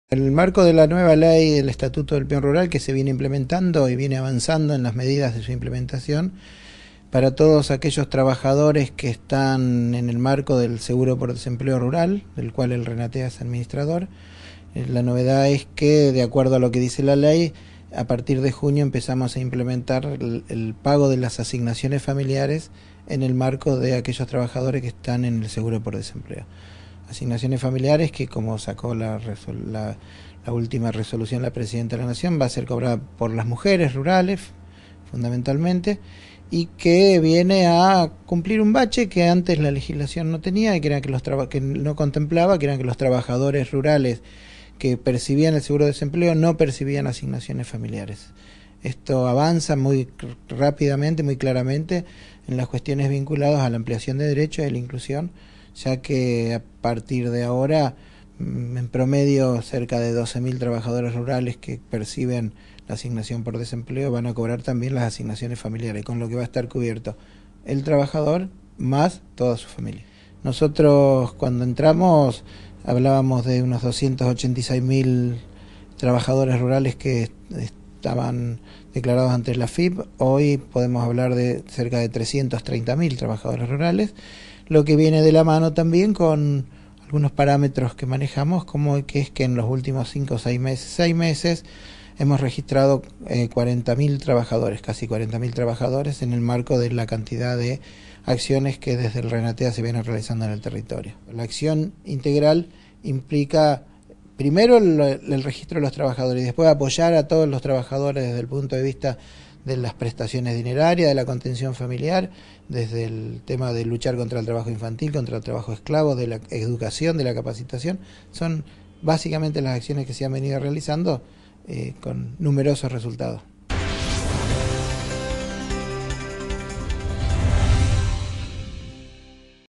Trabajadores agrarios comenzarán a percibir asignaciones familiares
El Registro Nacional de Trabajadores y Empleadores Agrarios (RENATEA) informó hoy que "a partir del mes de junio de 2013 los trabajadores del sector tendrán acceso a un nuevo beneficio: las asignaciones familiares que se percibirán junto a las prestaciones por desempleo".

Según el Director General del RENATEA, Guillermo Martini, "la cobertura está conformada por una prestación económica, prevención y mantenimiento de la salud del trabajador y su grupo familiar; capacitación, formación y provisión de medios que faciliten la reinserción laboral, y se suma ahora el pago de las asignaciones familiares".

"Esto avanza en las cuestiones vinculadas a la ampliación de derechos y a la inclusión y es posible a partir de la sanción de la Nueva Ley del Trabajo Agrario sancionada en diciembre de 2011", afirmó el funcionario en declaraciones a Télam.

Martini precisó que "el RENATEA es un organismo descentralizado del Ministerio de Trabajo de la Nación, cuya misión es garantizar la registración y otorgar protección a todos los trabajadores y trabajadoras en el marco del nuevo Régimen de Trabajo Agrario".

Sobre el avance en cuanto a la cantidad de trabajadores rurales registrados, el titular del RENATEA aseguró que "cuando entramos hablábamos de unos 286.000 que estaban declarados ante la AFIP y hoy ya podemos hablar de cerca de 330.000 trabajadores rurales".